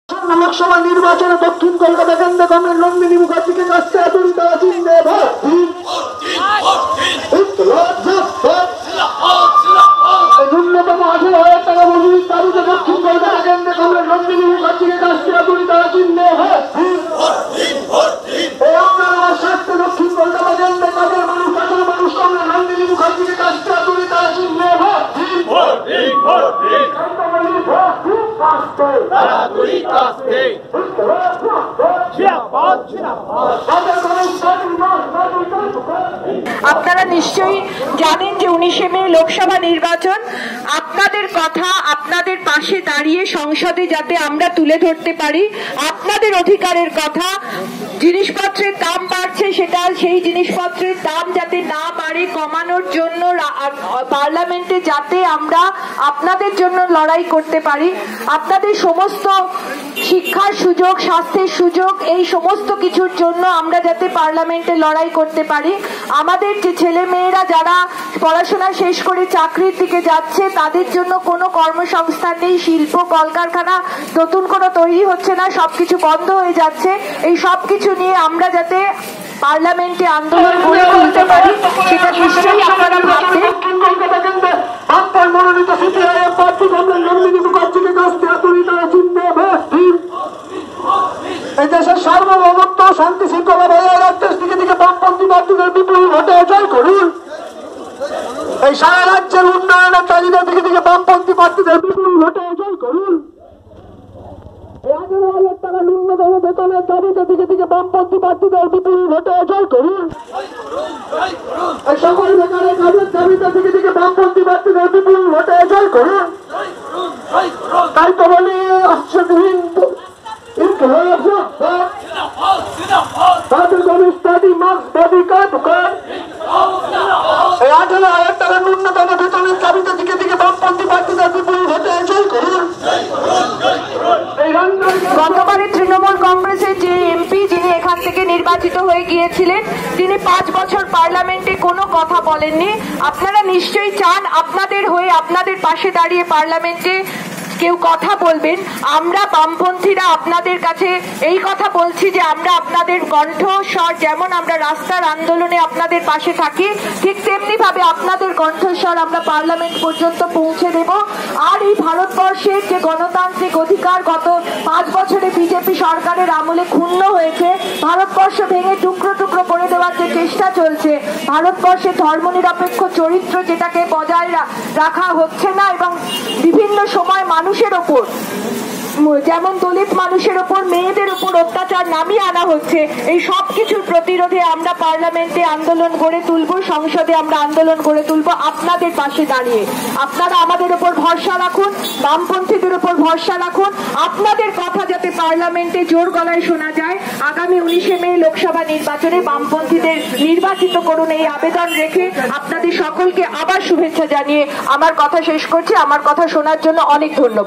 Nandini Mukherjee campaigns at South Kolkata | দক্ষিণ কলকাতায় নন্দিনী মুখার্জীর প্রচার | CPIM
Glimpses from the campaign of Left Front nominated CPI(M) candidate Dr. Nandini Mukherjee at South Kolkata